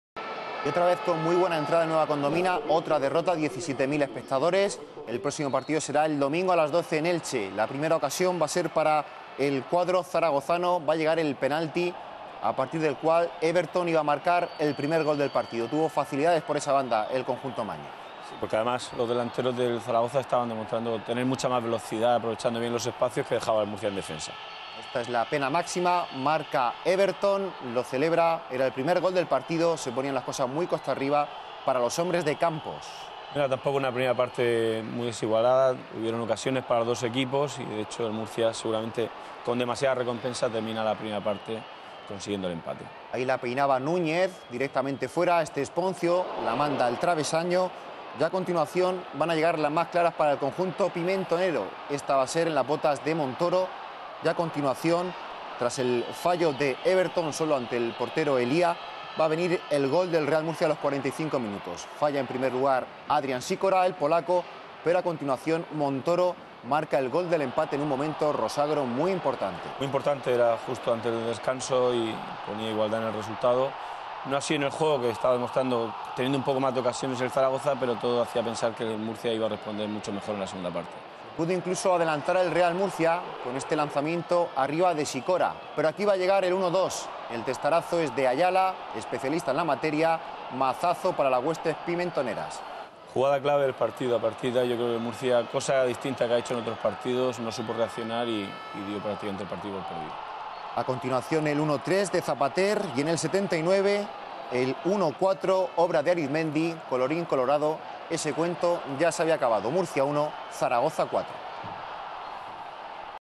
Jor. 26 - Real Murcia 1 - 4 Zaragoza
Partido correspondiente a la vigesimosexta jornada de la Liga Adelante Segunda División.